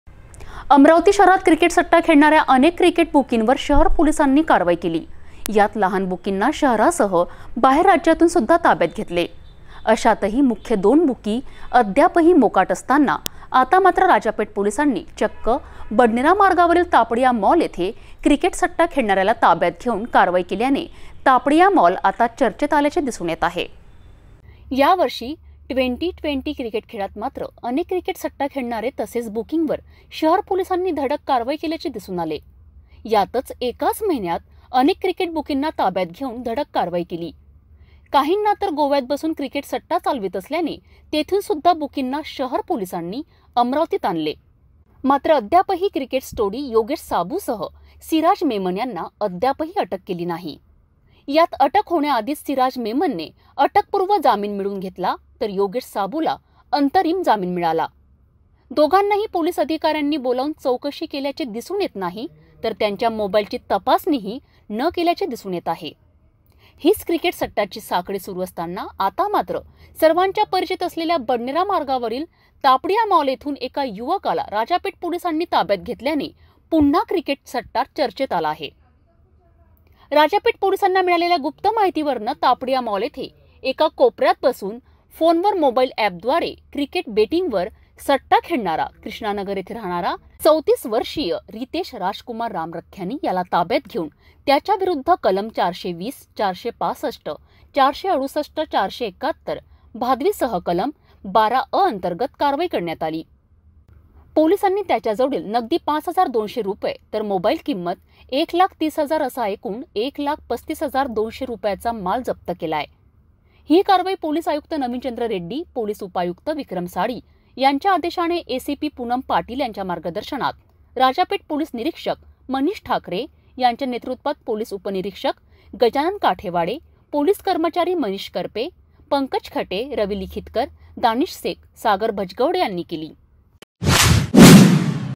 तापडियामॉल येथे क्रिकेटसट्टा खेळणाऱ्याला घेतले ताब्यात
कोपऱ्यात बसून मोबाईल अँप वर क्रिकेट बेटिंग वर खेळत होता सट्टा